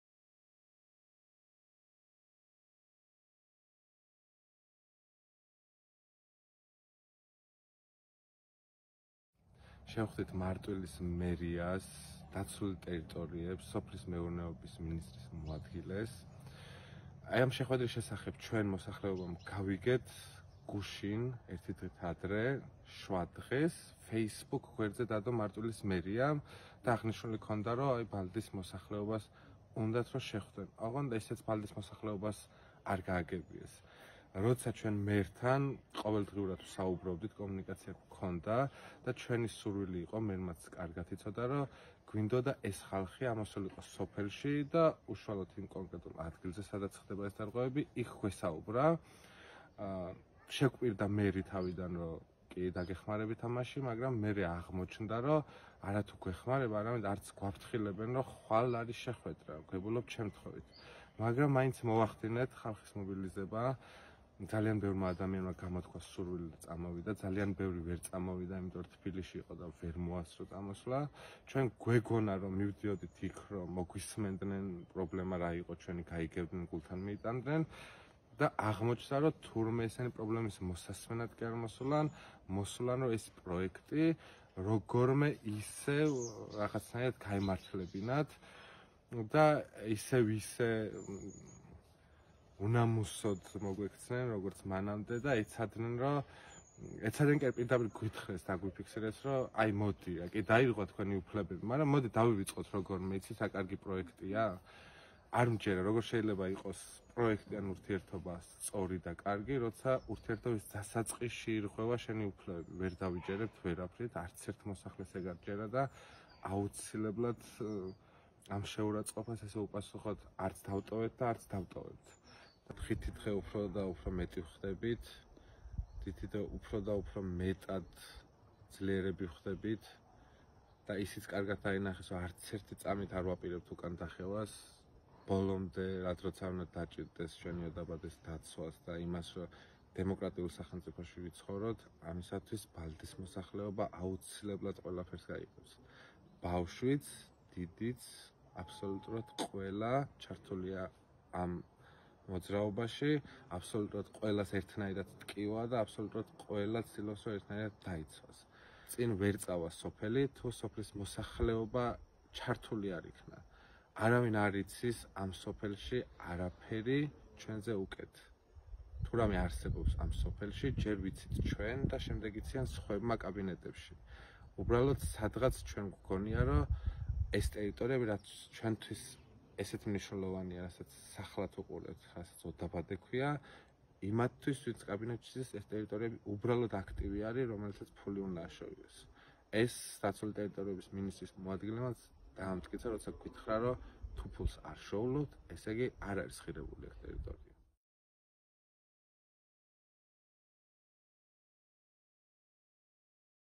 ბაჩო თვარაძე - ბალდის კანიონი
გასული წლის ბოლოს მარტვილის მუნიციპალიტეტში მდებარე ბალდის კანიონი, რომელსაც ბუნების ძეგლის სტატუსი აქვს, კერძო კომპანიას 40-წლიანი იჯარით გადაეცა. როგორც ადგილობრივები ამბობენ, ამ გადაწყვეტილების შესახებ ისინი ინფორმირებული არ ყოფილან და კანიონის კერძო ინვესტორისთვის გადაცემის შესახებ მხოლოდ მას შემდეგ შეიტყვეს, რაც კომპანიამ ადგილზე სამუშაოები დაიწყო და კანიონის ტერიტორია შემოღობა.

ამ ვიდეოში მარტვილის მუნიციპალიტეტის სოფელ ბალდის მცხოვრები, ბაჩო თვარაძე გვიყვება